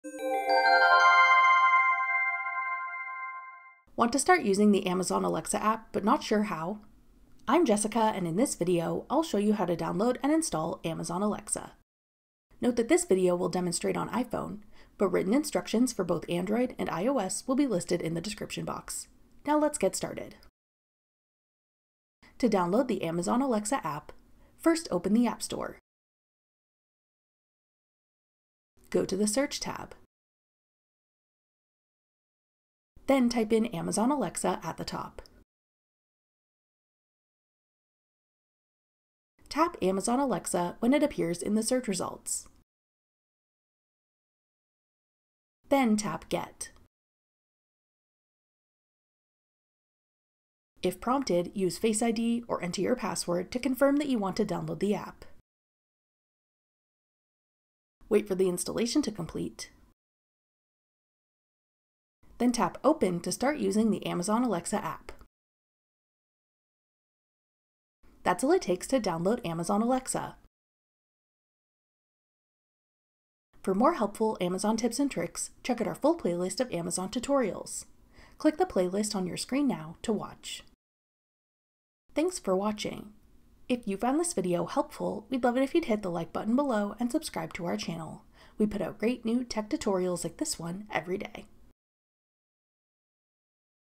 How to Download & Install Amazon Alexa App in 2026
Want to start using the Amazon Alexa app but not sure how? Watch this video to learn how to download the Alexa app.

To download & install Amazon Alexa on an iPhone or iPad:
Go to the Search tab, and search “Amazon Alexa” at the top.
Tap Amazon Alexa in the search results.
Wait for the download to finish, then tap Open to start using the Amazon Alexa App.

To download & install Amazon Alexa on an Android device:
Tap the search bar at the top, and type in “Amazon Alexa.”